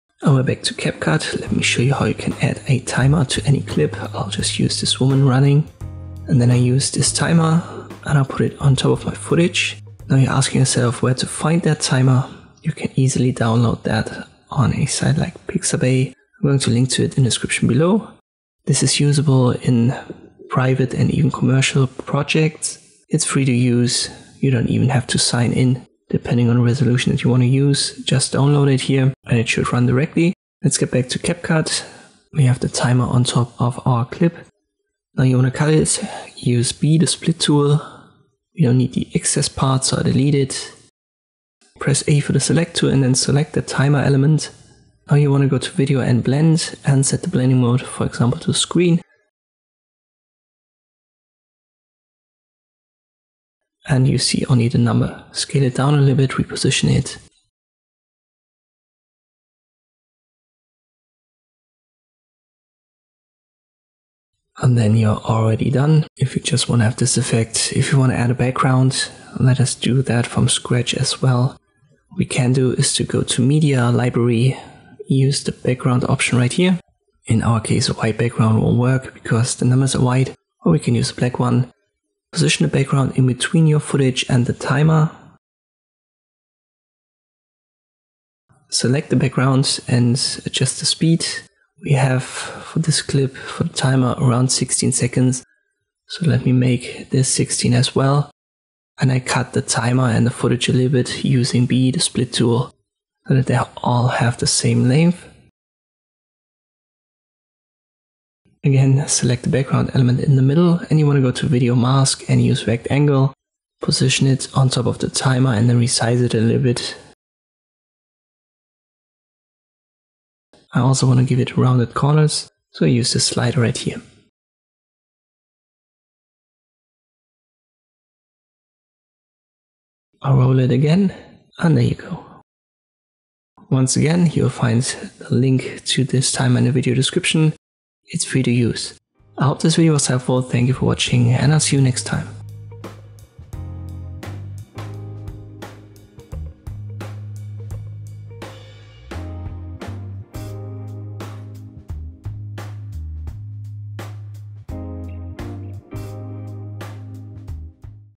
Create a Timer Effect in CapCut
In this tutorial I'll show you how to create a timer effect in CapCut.